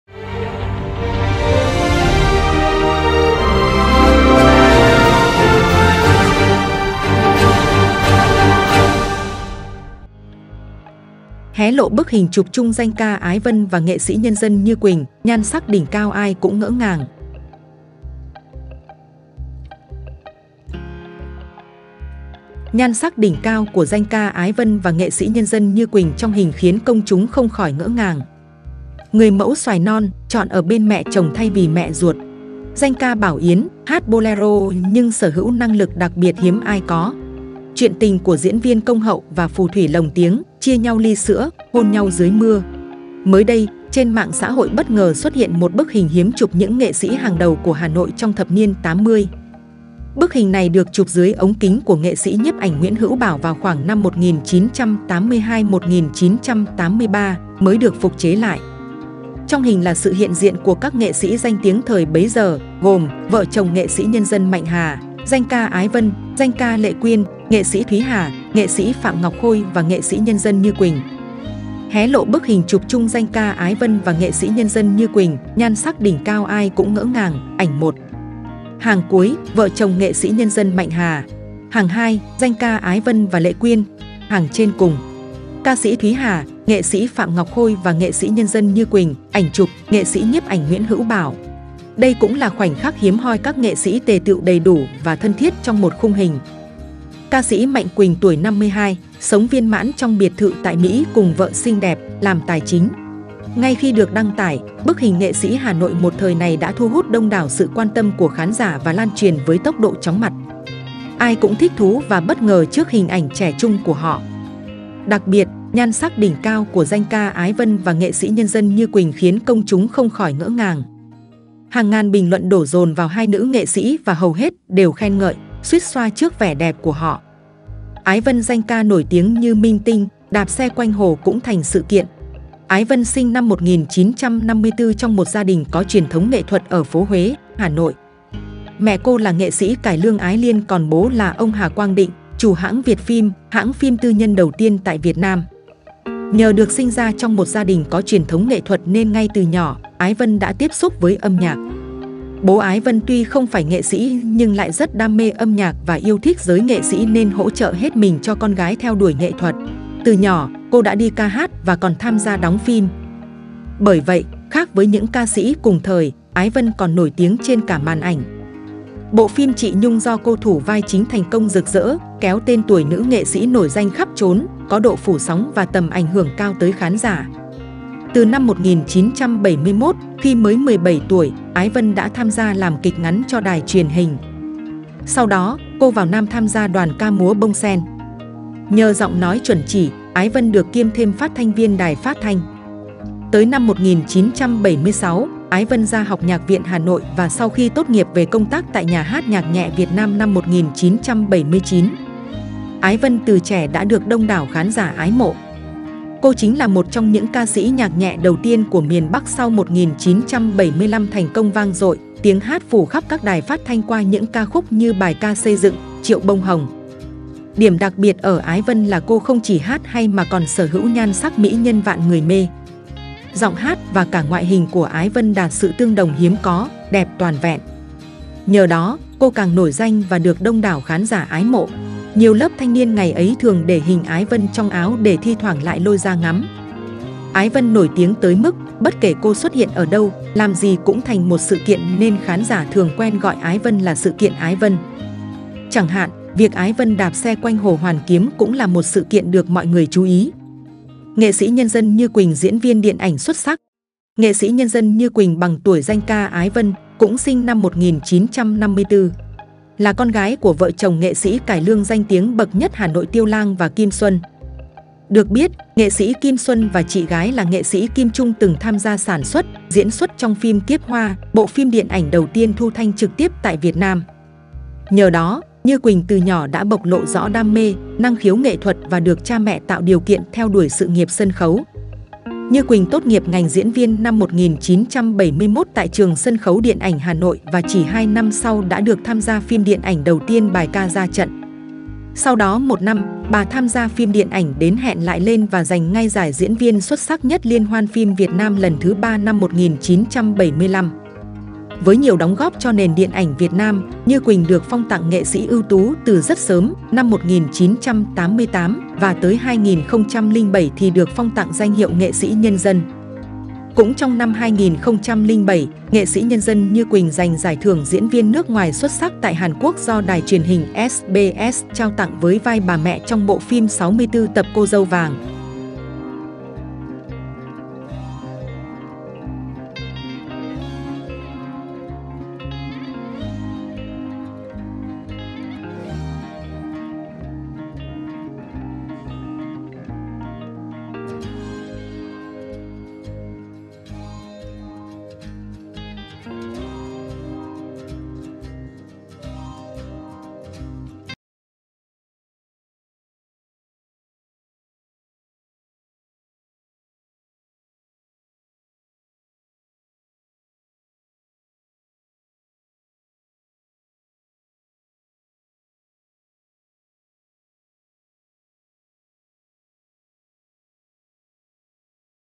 Hé lộ bức hình chụp chung danh ca ÁI VÂN và NSND NHƯ QUỲNH - Nhan sắc đỉnh cao ai cũng ngỡ ngàng
Mới đây, trên mạng xã hội bất ngờ xuất hiện một bức hình hiếm chụp những nghệ sĩ hàng đầu của Hà Nội trong thập niên 80.

Bức hình này được chụp dưới ống kính của nghệ sĩ nhiếp ảnh Nguyễn Hữu Bảo vào khoảng năm 1982 – 1983, mới được phục chế lại. Trong hình là sự hiện diện của các nghệ sĩ danh tiếng thời bấy giờ, gồm: Vợ chồng NSND Mạnh Hà, danh ca Ái Vân, danh ca Lệ Quyên, nghệ sĩ Thuý Hà, nghệ sĩ Phạm Ngọc Khôi và NSND Như Quỳnh.